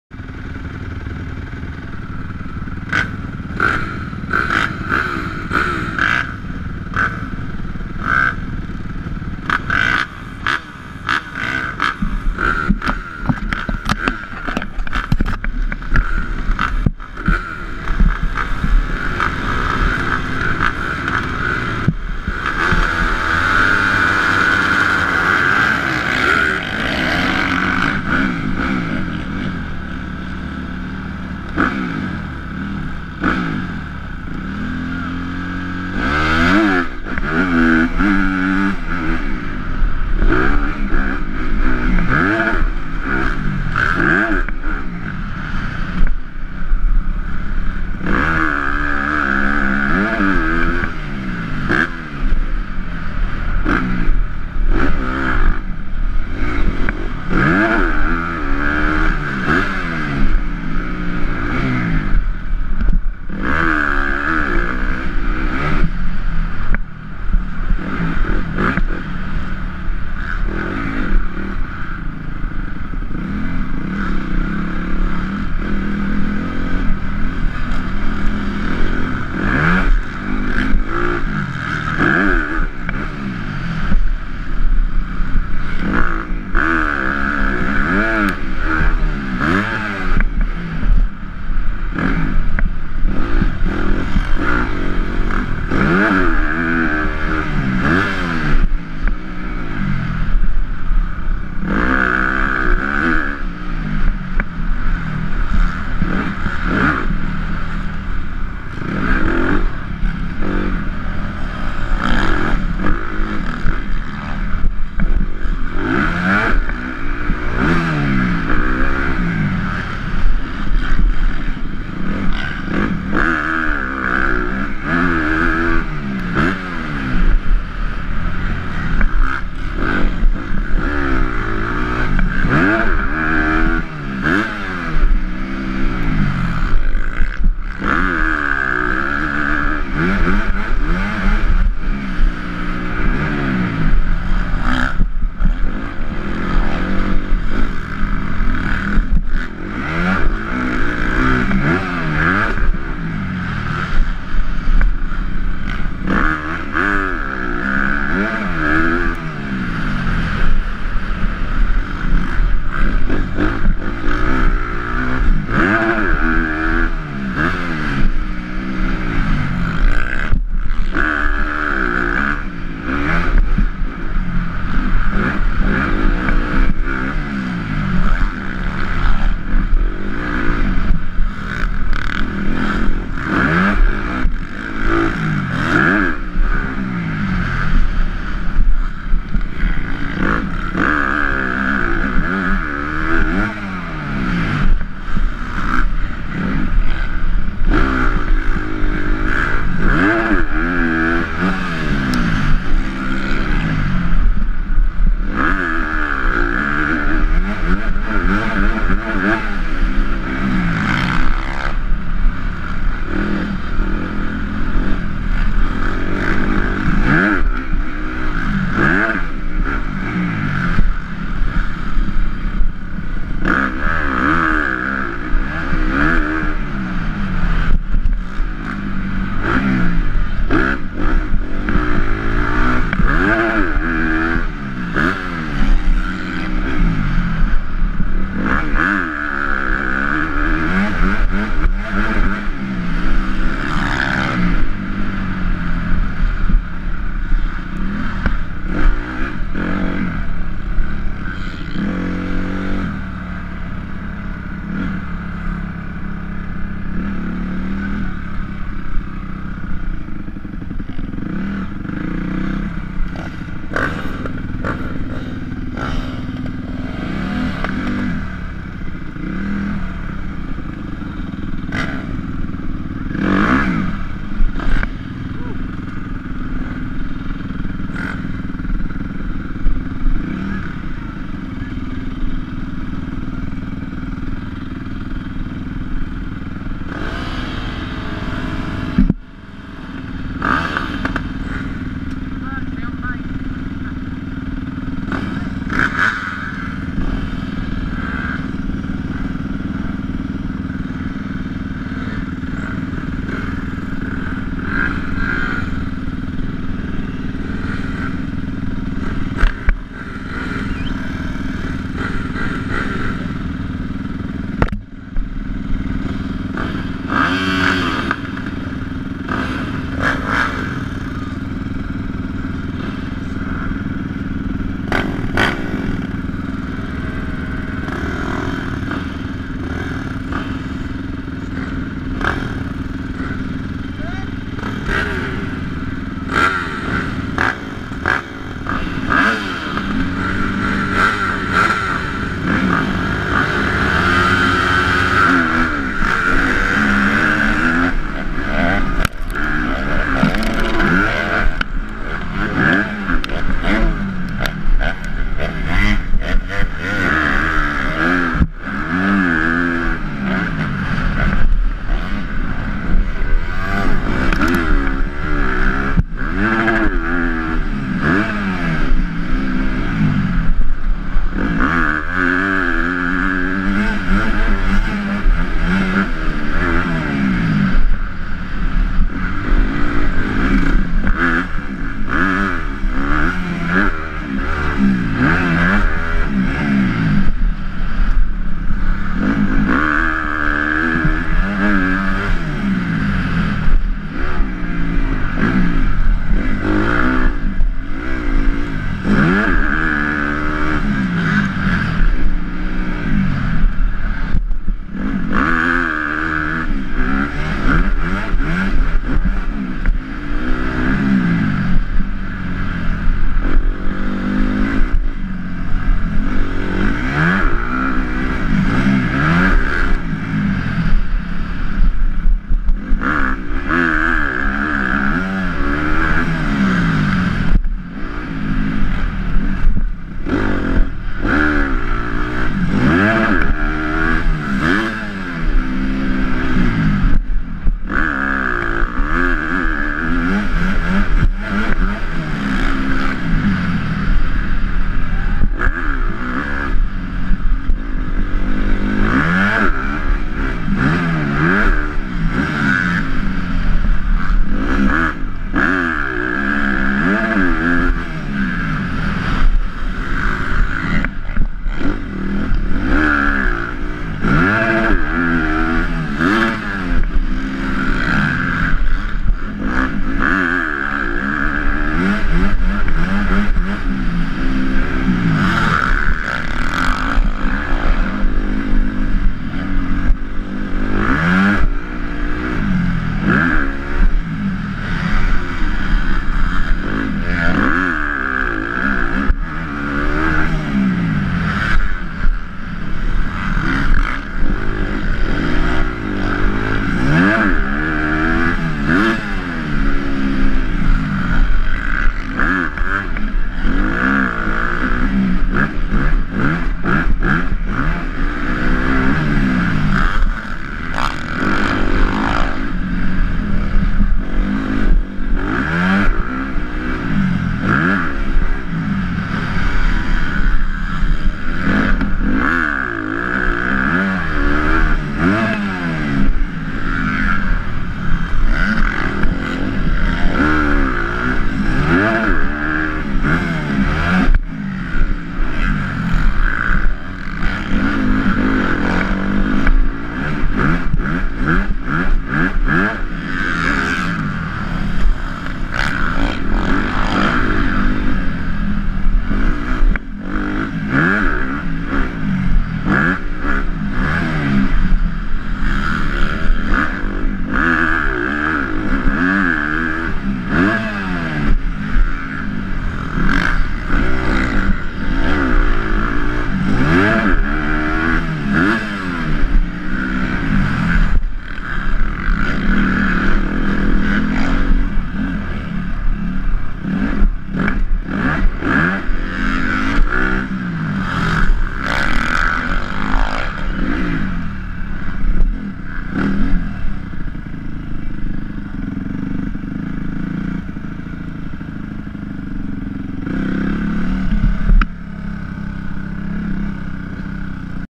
Ty edmondson Taft arenacross
Ty having fun in a vet 1st moto and then putting it down in his 250-450 novice class.